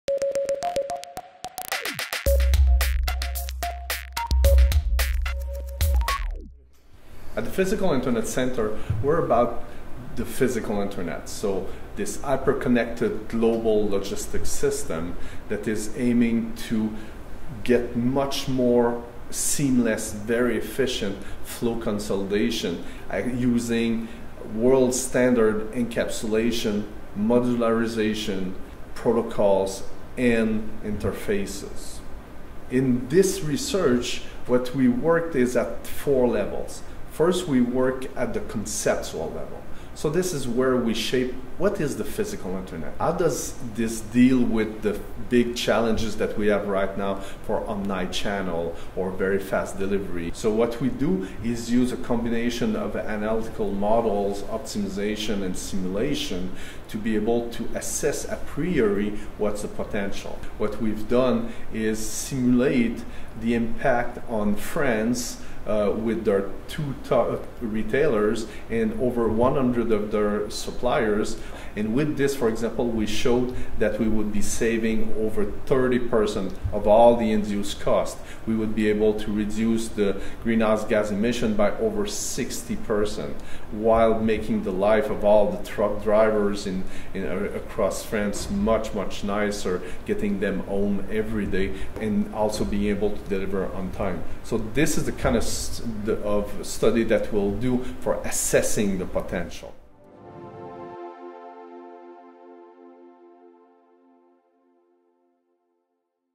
Research Spotlight: Physical Internet Center
Dr. Benoit Montreuil explains Georgia Tech's initiatives to develop the cutting edge knowledge required to design and operate the globally emerging "Physical Internet".